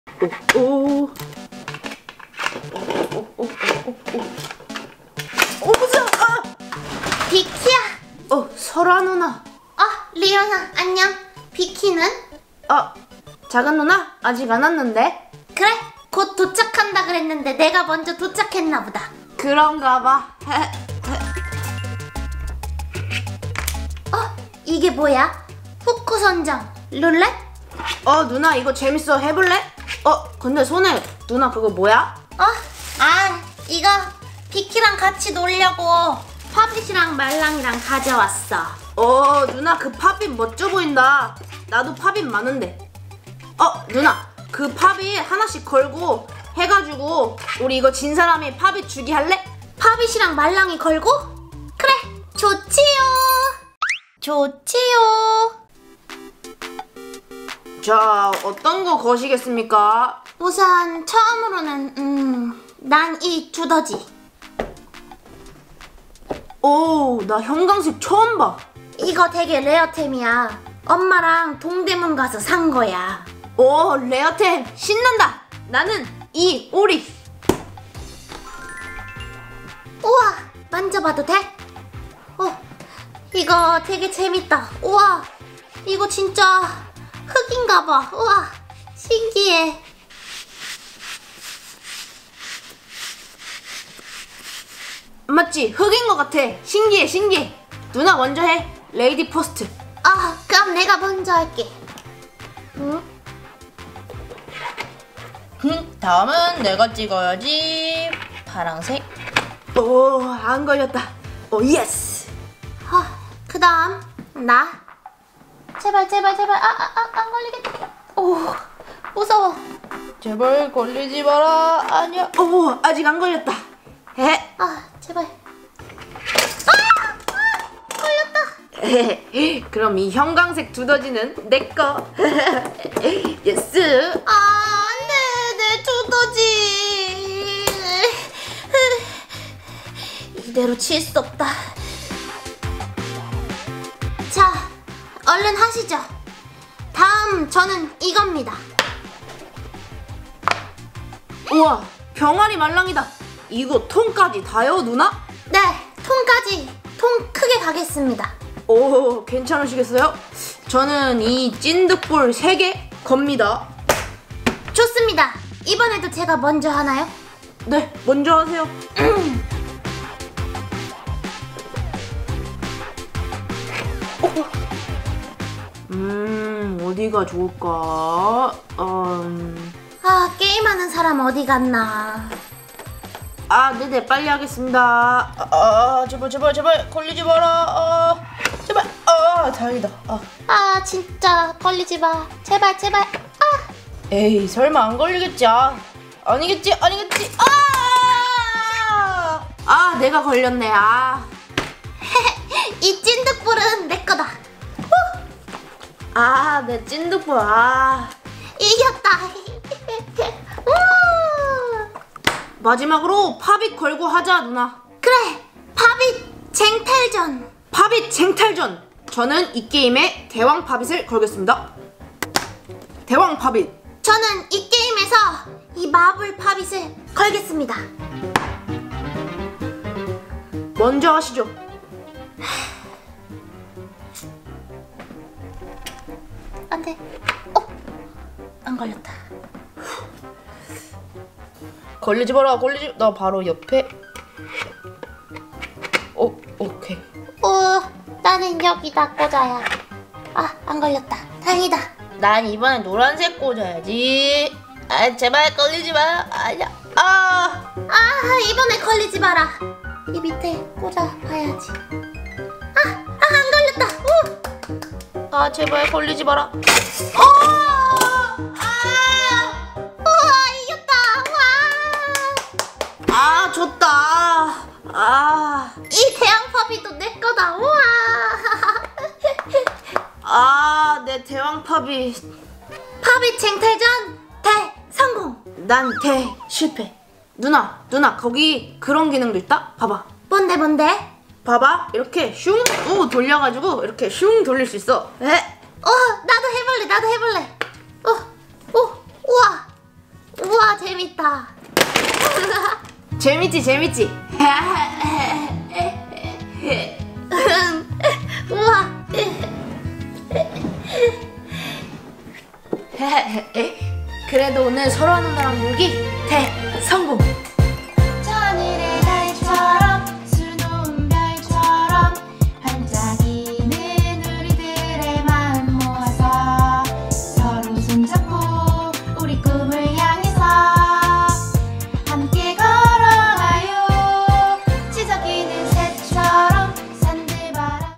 팝잇 쟁탈전- 좋아하는 누나와의 대결, 최후의 승리자는? [오마이비키]
게임에서 이겨
팝잇을 가져갈
행운의 주인공은?